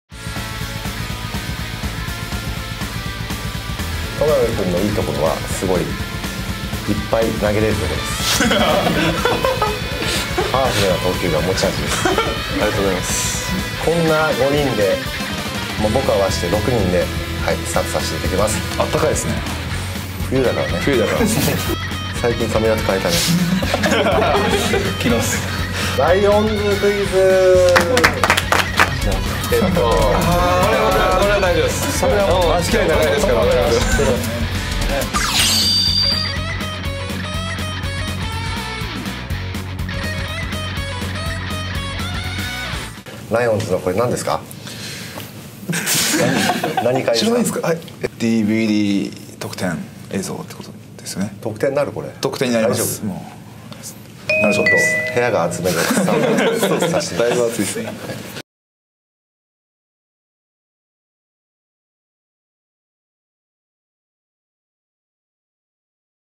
FC早期入会特典「オリジナルDVD」サンプル動画を公開！
2015年3月31日（火）までに、ライオンズファンクラブに入会された方にもれなくプレゼントする「ファンクラブオリジナルDVD」のサンプル動画を公開！
恒例の「選手座談会」では、大物ゲスト・球団OB石井一久さんをMCとしてお招きし爆笑トーク満載です!!

ファンクラブに入会して限定映像満載のオリジナルDVDをゲットしよう！